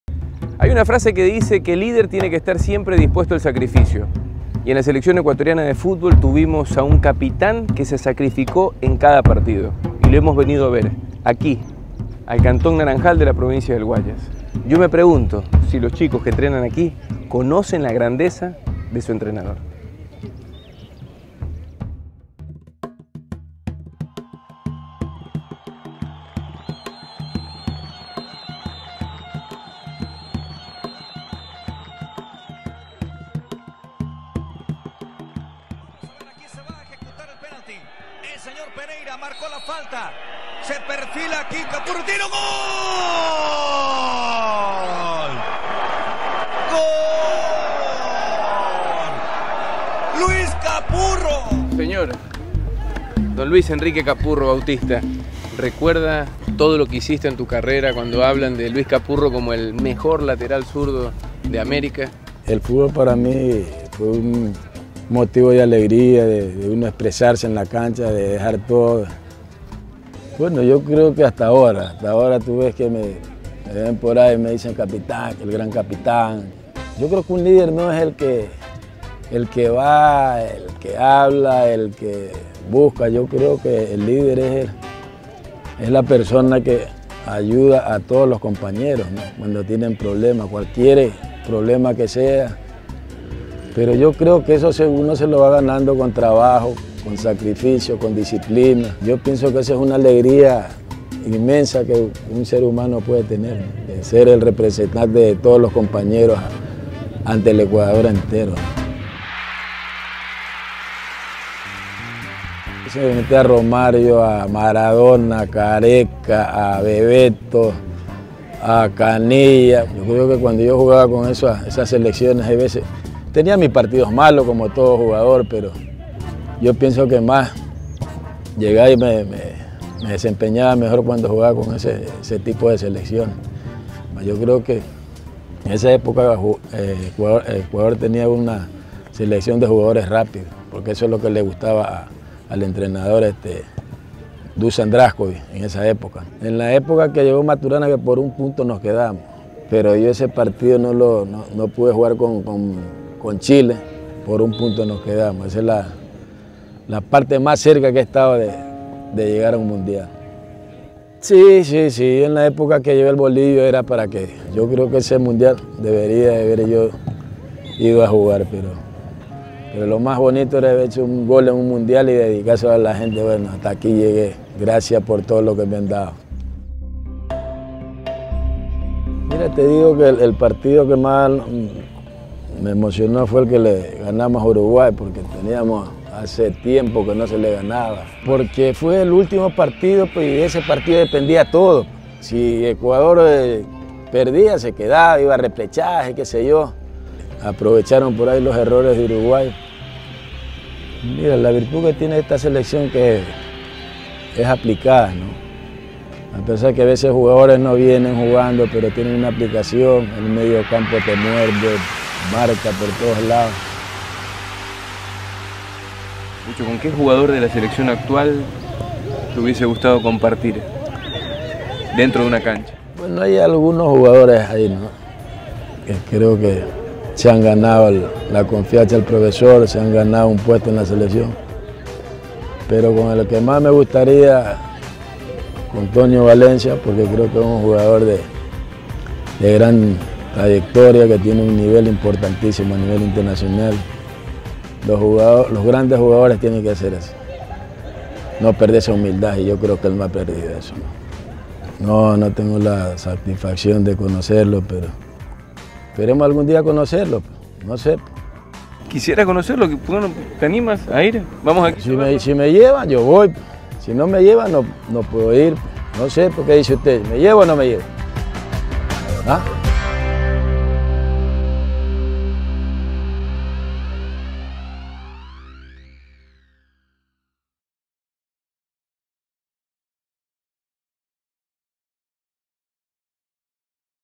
Inmortales - El Gran Capitán
Lo que hace grande a una leyenda no es su recorrido ni la historia que deja atrás. Es su legado que supera al tiempo, el que inspira a muchos a creer en lo imposible

Luis Capurro, ¨El gran capitán¨, el mejor de América en su puesto por muchos años. Lateral izquierdo de clase mundial, es parte de Inmortales.